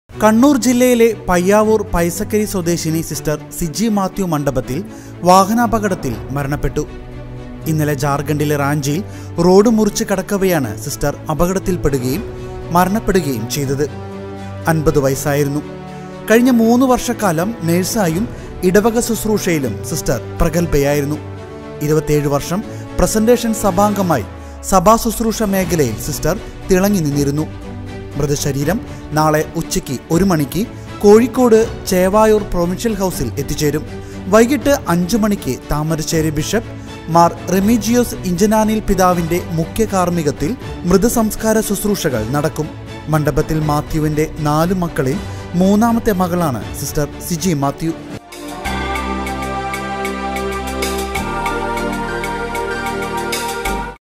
റോഡ് മുറിച്ചുകടക്കവേ വാഹനാപകടം മലയാളികന്യാസ്ത്രീ അന്തരിച്ചു|ACCIDENT|NUN|CHURCH|FUNERAL|GOODNESSNEWS
റോഡ് മുറിച്ചുകടക്കവേ വാഹനാപകടം, മലയാളി കന്യാസ്ത്രീ അന്തരിച്ചു|ACCIDENT|NUN|CHURCH|FUNERAL|GOODNESS NEWS